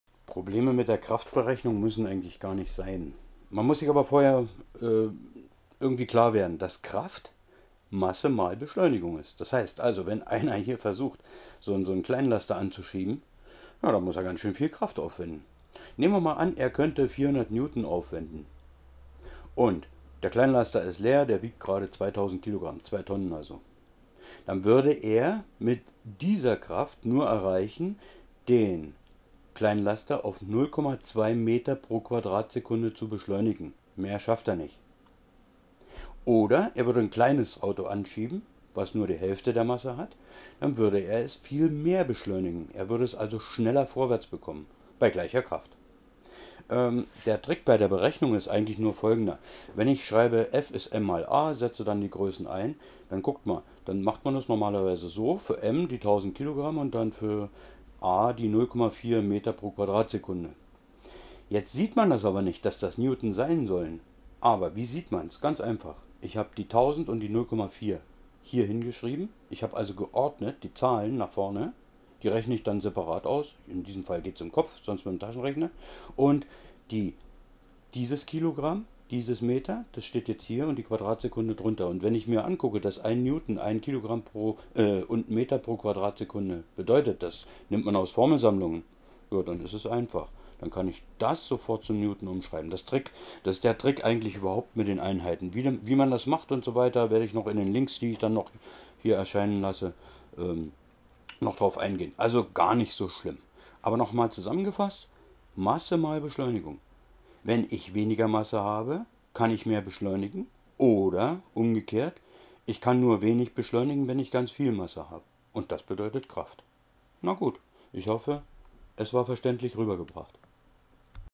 Kraft, was ist das? Warum berechnet man Kräfte mit F=m*a ? Warum dann die Einheit "Newton"?
Bitte, helfen Sie uns Endlich wieder (Sapere aude!) Genau das wird kurz und einfach erklärt, denn es IST einfach. Warum? Einfach mal gucken.